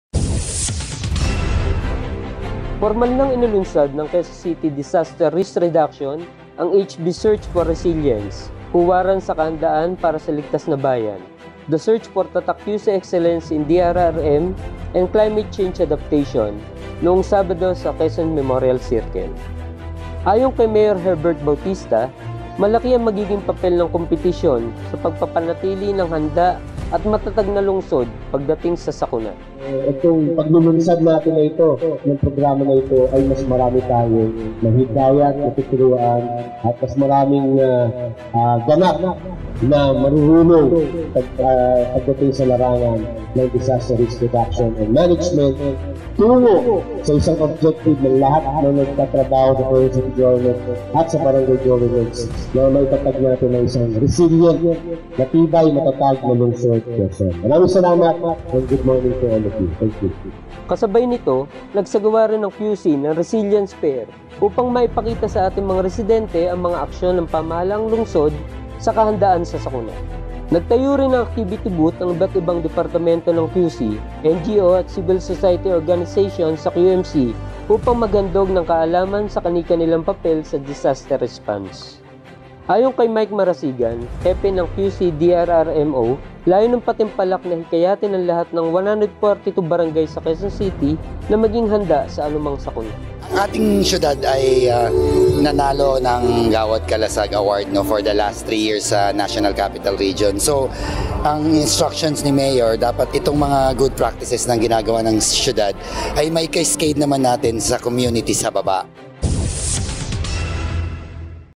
HB Search for Resilient Barangays launched
Pormal nang inilunsad ng QC Disaster Risk Reduction ang HB Search for Resilience: Huwaran sa Kahandaan para sa Ligtas na Bayan, The Search for tatak Kyusi Exccellence in DRRM and climate change adaption noong sabado sa Quezon Memorial Circle.